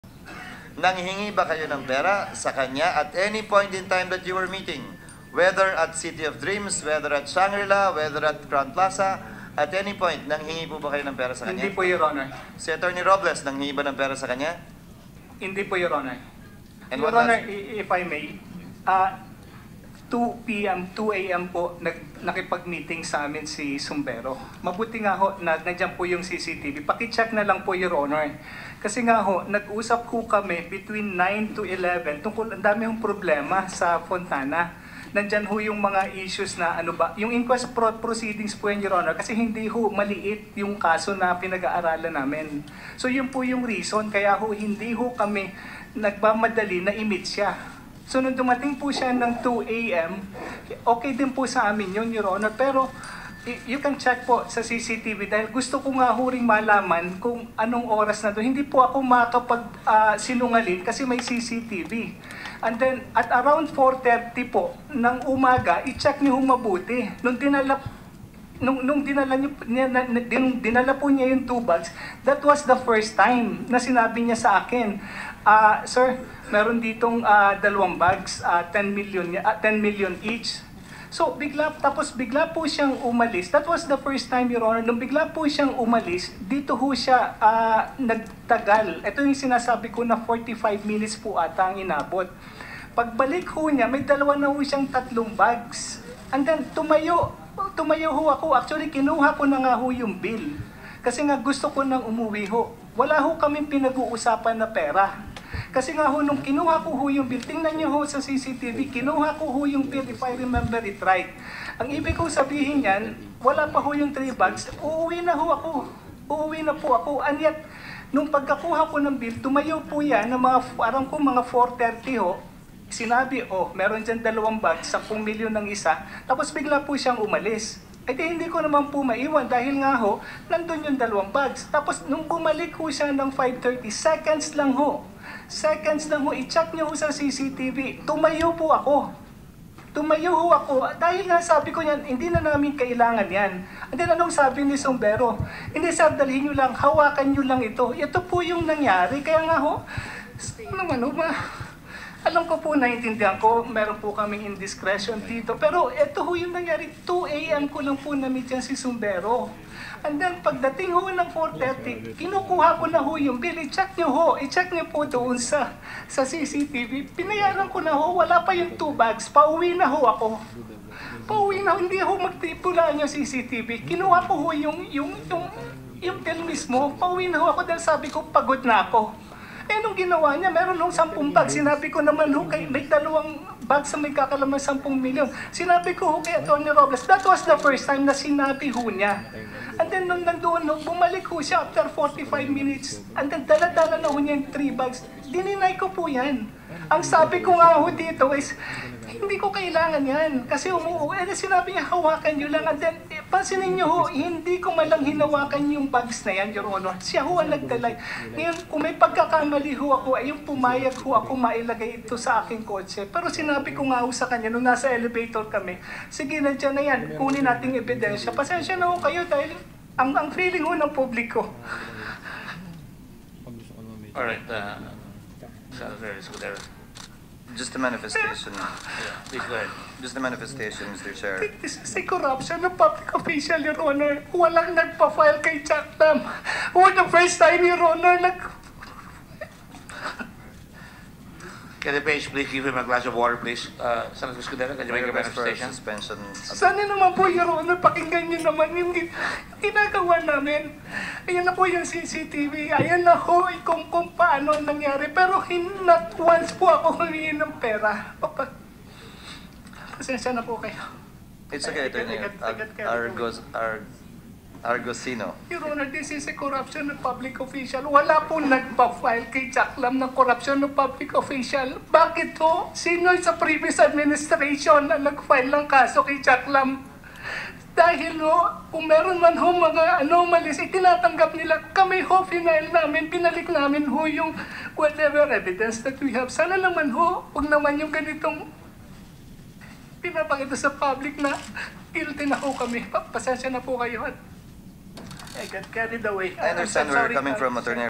Argosino turns emotional, breaks down
More videos on Rappler Follow Rappler for the latest news in the Philippines and across the globe. Former Immigration Deputy Commissioner Al Argosino turns emotional at the Senate hearing on Jack Lam and the alleged bribery in the Bureau of Immigration